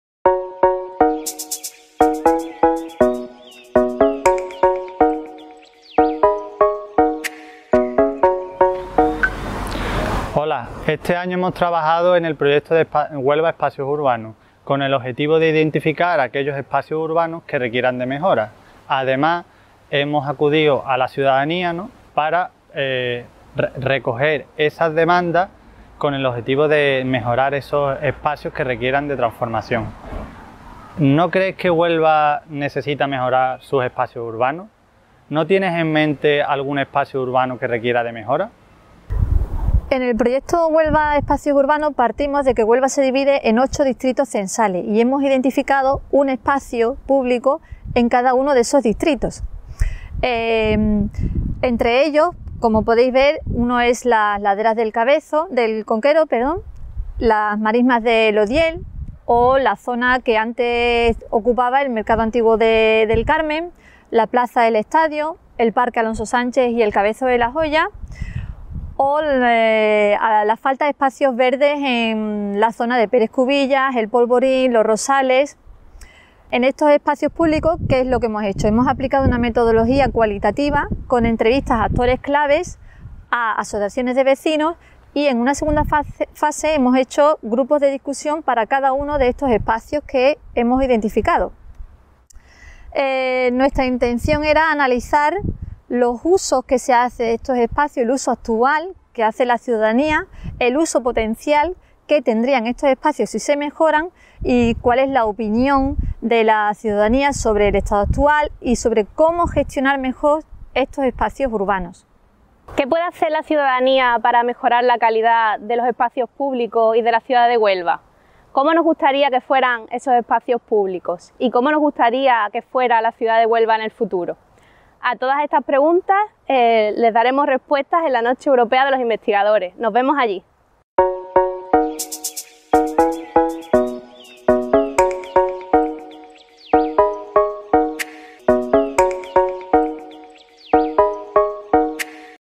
Promo 'Huelva Espacios Urbanos' - La Noche Europea de los Investigadores 2021
Esta actividad consiste en realizar una visita o ruta guiada sobre los espacios públicos urbanos que han sido identificados en el proyecto de investigación "Huelva Espacios Urbanos".